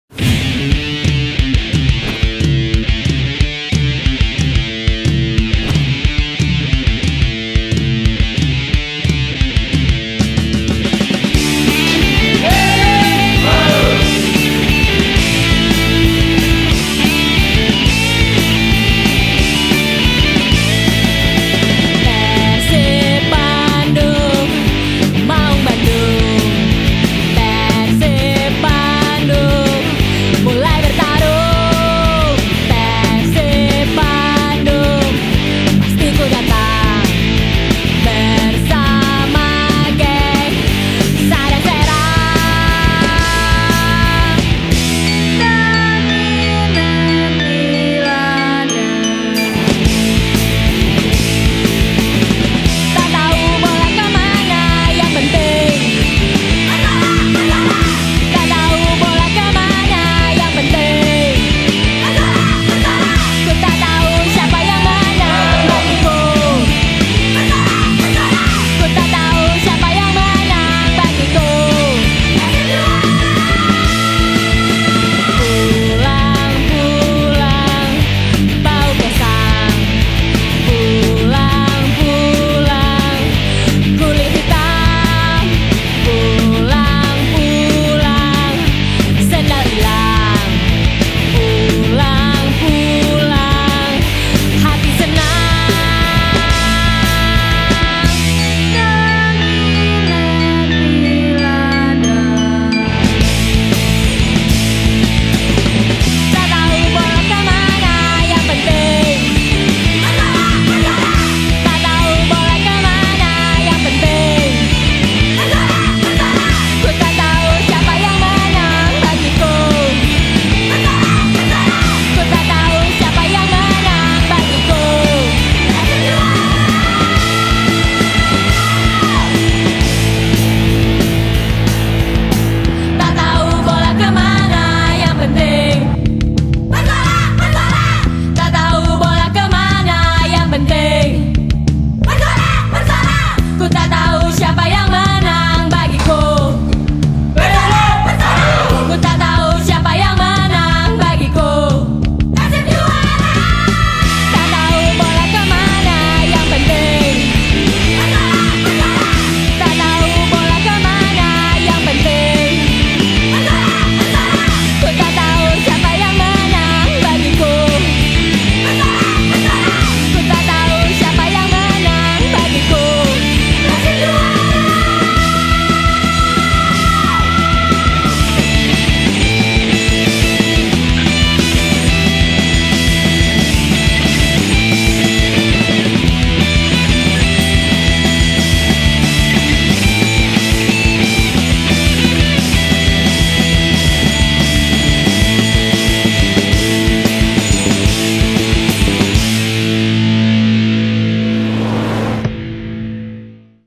AWAY DAY MAKASSAR | Gojek Traveloka Liga 1 2017 | Match 15 Oktober 2017
Perjalanan dan kegiatan Tim PERSIB dalam menjalani laga tandang melawan PSM di Makassar.